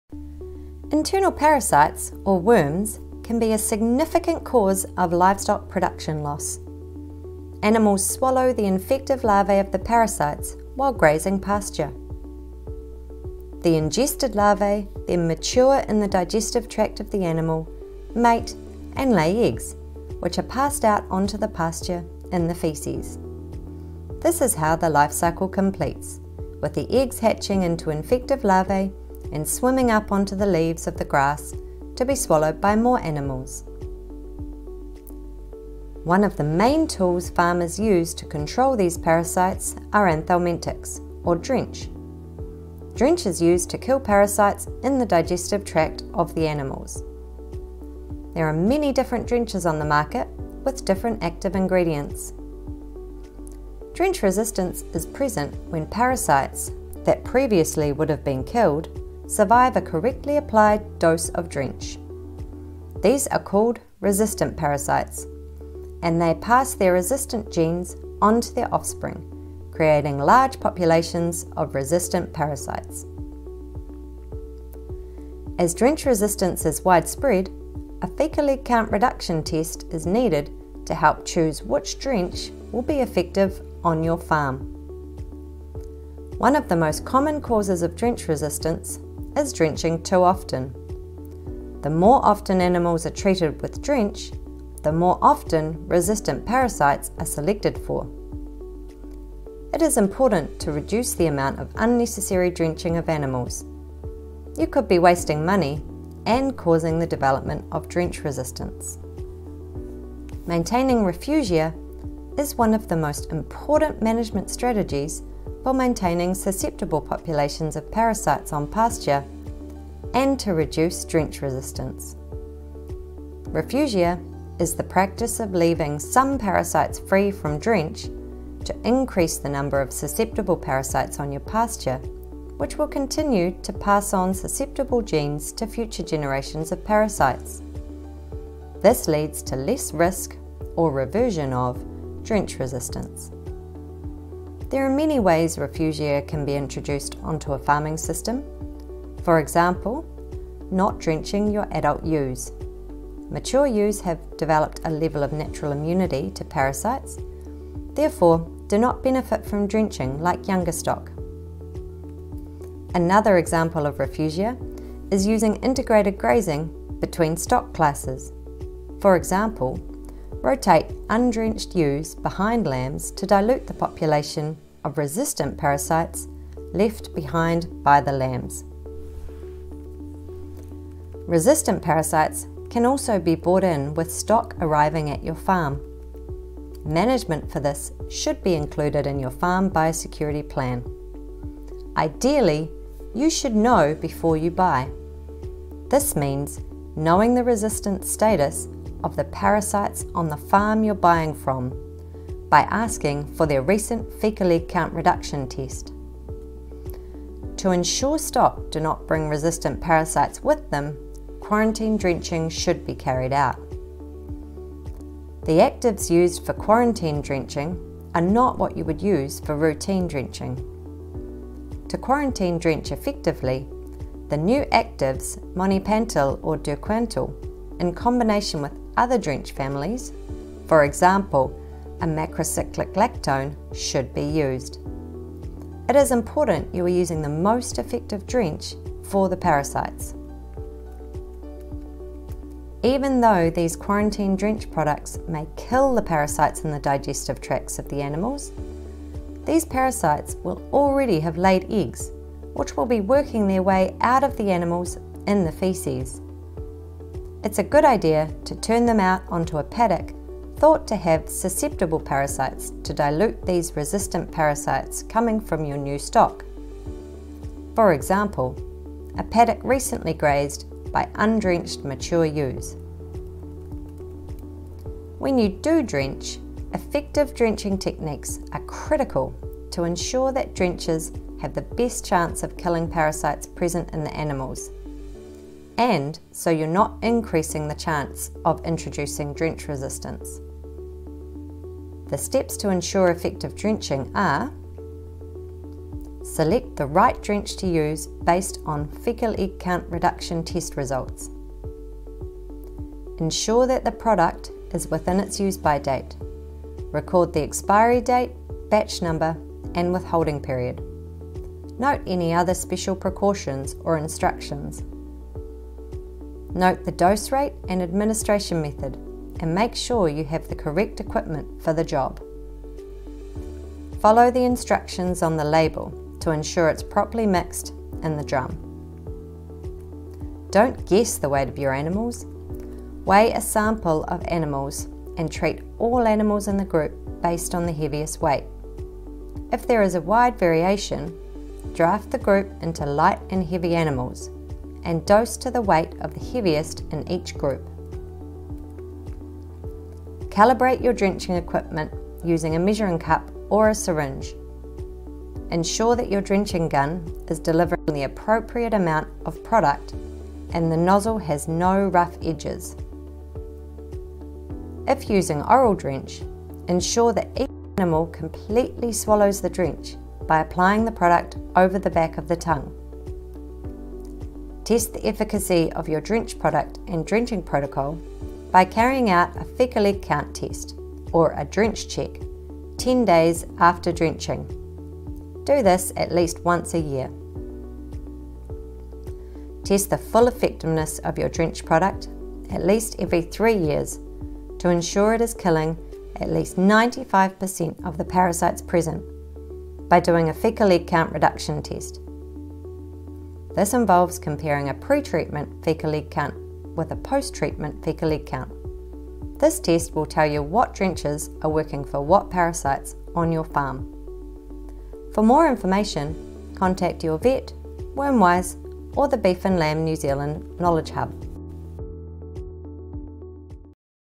Parasites 101
Internal parasites, or worms, are a significant cause of livestock production loss. Anthelmintics, or drench, is the most common tool farmers use to control parasites. However, parasites are becoming increasingly resistant to drench with drenches failing on an increasing number of farms in New Zealand. Watch this video to get an overview of parasites, some examples of how to delay the development of drench resistance. Thanks to Teagasc for the inspiration behind this video.